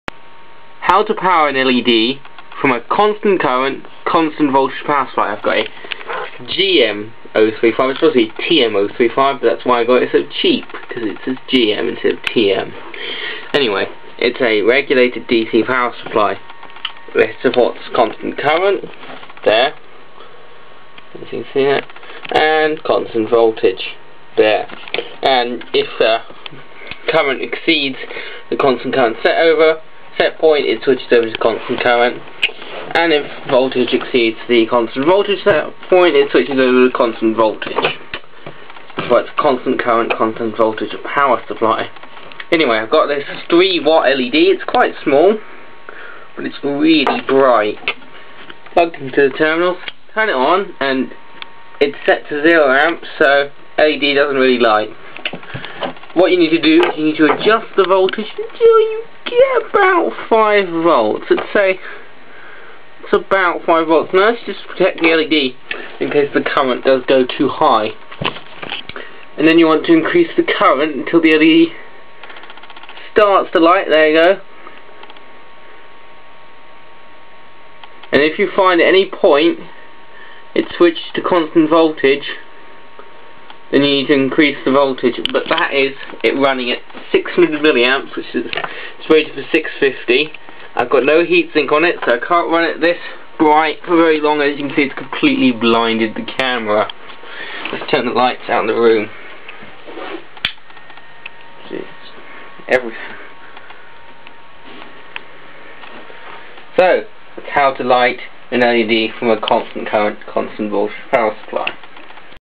Powering an LED from a CC/CV (constant current / constant voltage) power supply
How to power an LED (a 3W pure green) from a CC/CV (constant current / constant voltage) power supply. In this video I show a beefy mains operated supply but there is no reason it could not be done with a smaller unit, the principle of operation is identical. Sorry about the shaky camera work but I was trying to hold the camera while adjusting settings on the supply. I'll try and find a tripod next time.